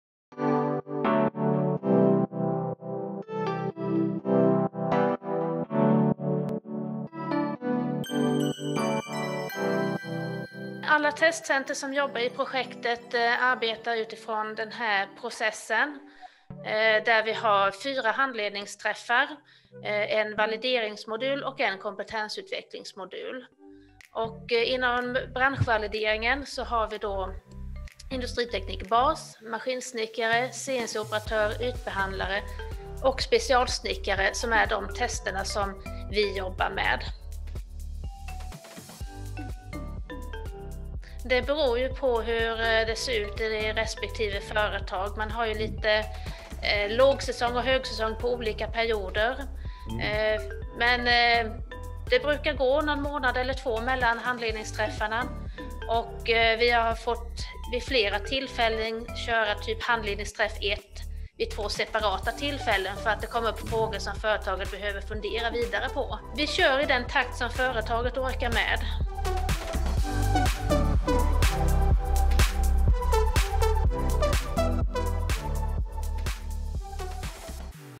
Vilka insatser ingår i projektet och vilken hjälp får man som företag?
Bodil Svensson, Träcentrum berättar om insatserna; Handledning, Branschvalidering och Kompetensutveckling.